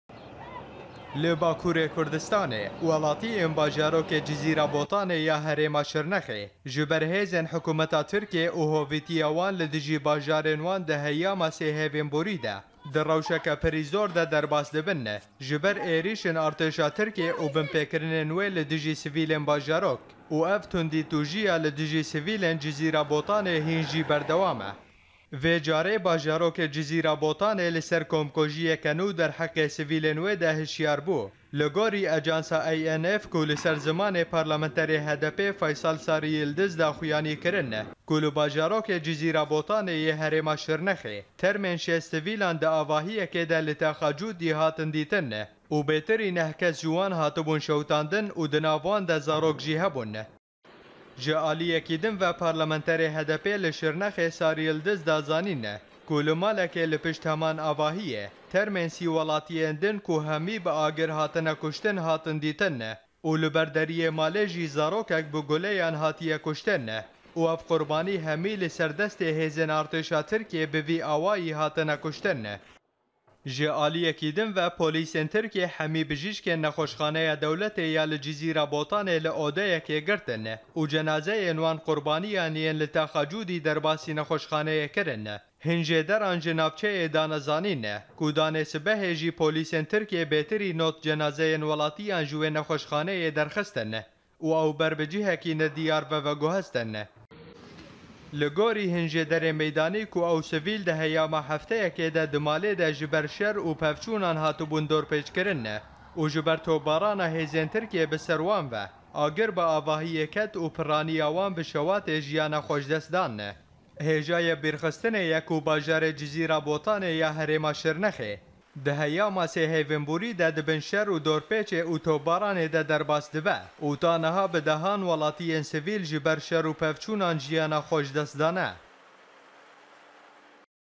HDPê: Li Cizîra Şirnexê 30 sivîl bi şewatê hatine kuştin
30 kes ji ber êrîş û şewata agirê Tirk ve Duh hatin kuştin, Faysal Sariyildiz.. Parlemanterê Partiya Demokratîk a Gelan HDPê li navçeya Cizîra Şirnexê ragihand ku termê bêtirî 30 welatiyên sivîl ku bi şewatê hatine kuştin di hindirî avahiyekê de hatin dîtin .. û bêtirî 60 sivîlên din jî di hindirî malekê de hatine kom kirin û ji aliyê Artêşa Tirkiyê ve hatine kuştin